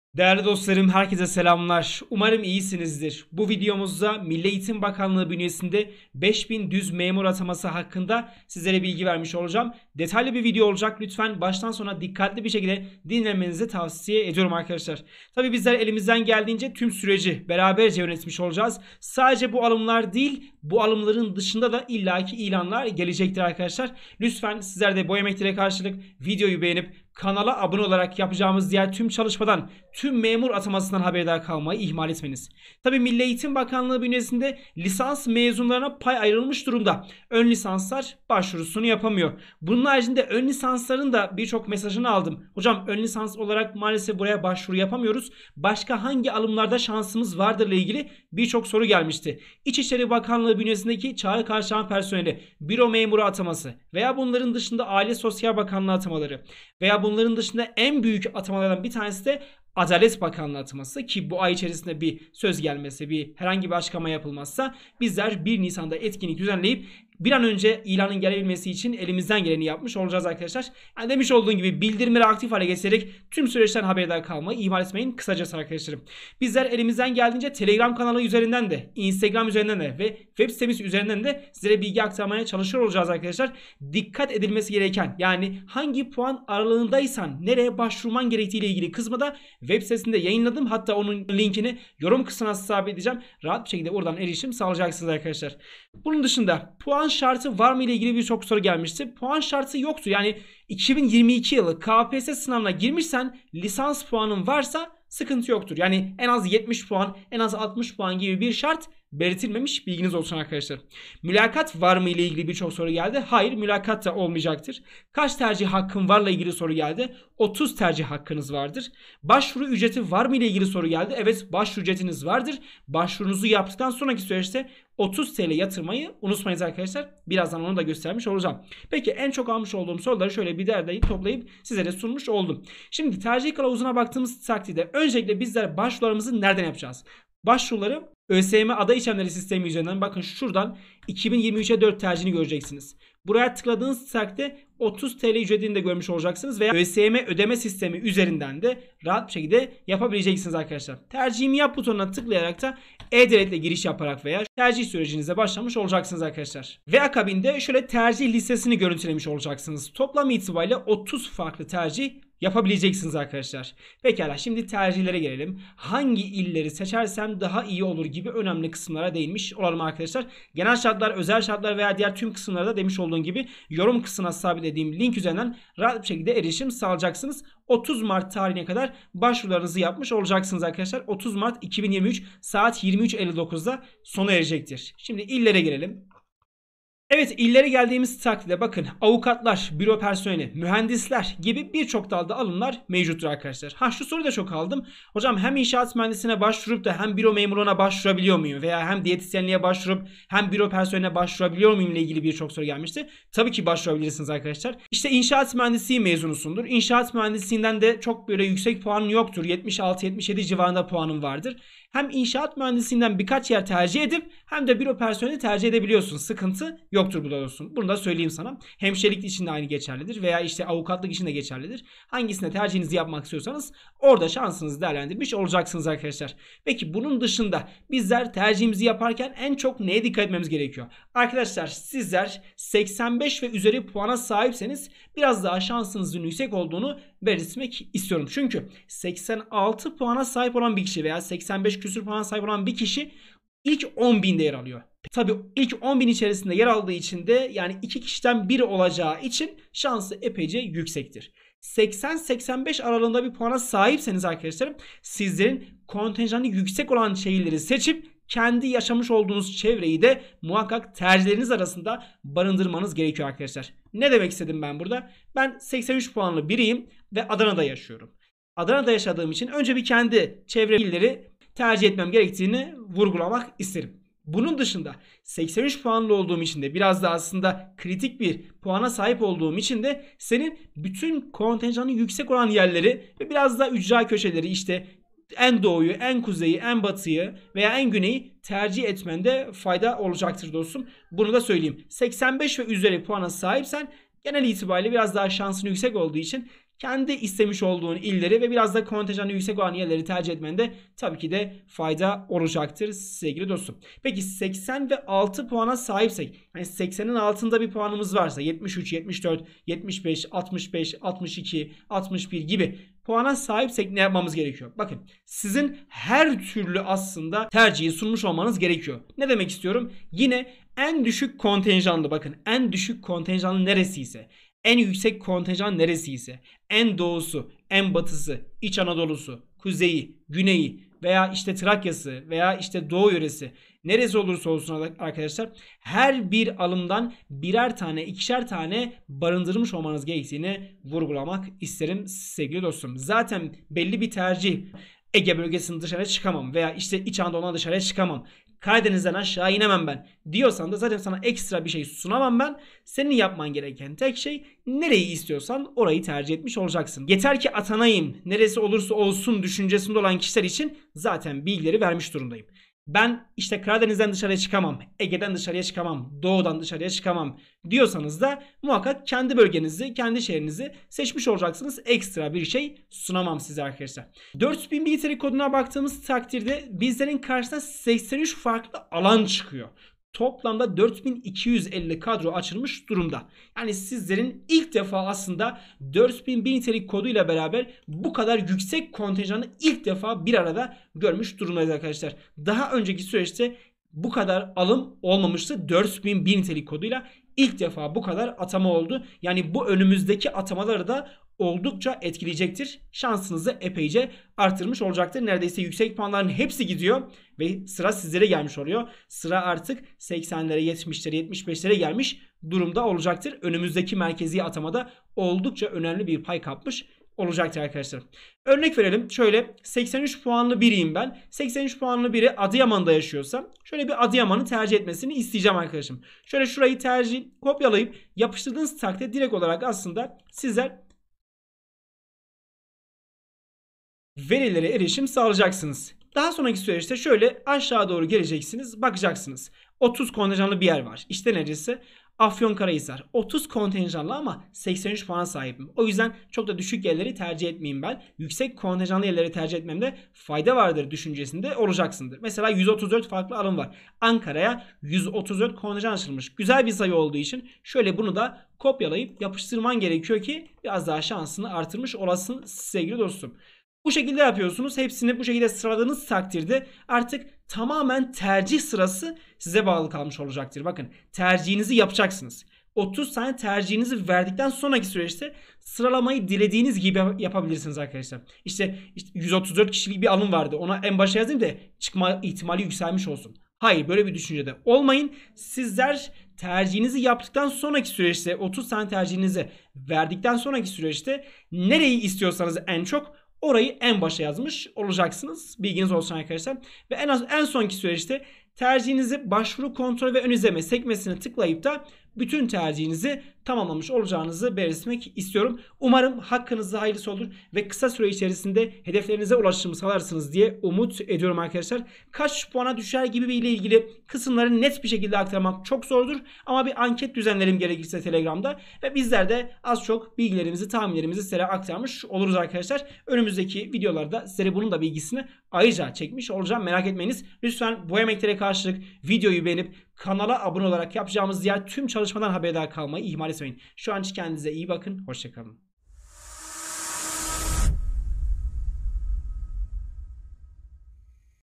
2023/4 ÖSYM MERKEZLİ ATAMA MEB 5000 MEMUR ALIMI DOĞRU TERCİH NASIL YAPILMALIDIR✅ EN DÜŞÜK PUAN KAÇ ✅
5 BİN MEMUR ATAMASI İÇİN DOĞRU TERCİH ✅